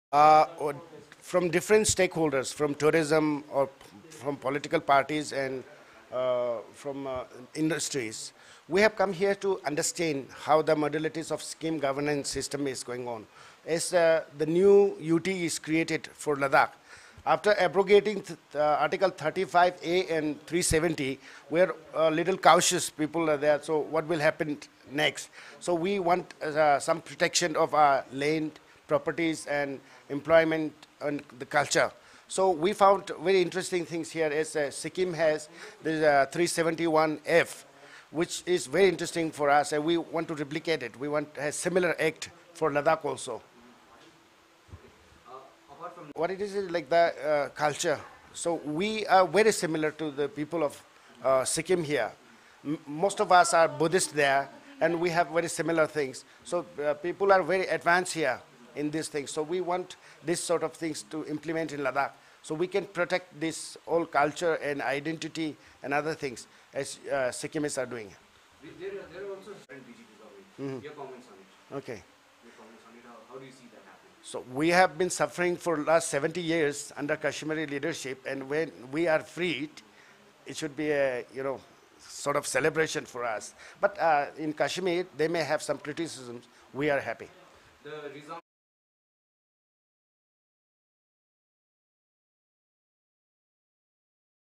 A team from Ladakh under the leadership Dorje Angchok.
A team from Ladakh under the leadership Dorje Angchok. President, BJP, Leh-Ladakh is in tour to Sikkim to understand the set up in Sikkim so that they can adopt in thier newly made union territory.

During their 4 day long visit to state the team interacted with the bureaucrats of the state and they appreciated how Article 371 F has protected the Sikkimese people and land also after their visit they have made their mind to make a provision have  similar type of safeguard to their Union Territory also  once they starts formulating the set for Union Territory. They are trying understand the modalities.

The team comprises from all the stakeholders including from various fields. Other members are Tashi Gyalson (Advocate), Vice-President, BIP, Leh-Ladakh 3. Dr. Skalzang Dorjey, Adviser, Panchayat Cordination committee, LehLadakh 4. Nawang Thinles, Chairman, Confederation of Indian Industry (CU). Leh-Ladakh, Deleks Namgyal, General Secretary, All Ladakh Tour Operator Association, Leh-Ladakh.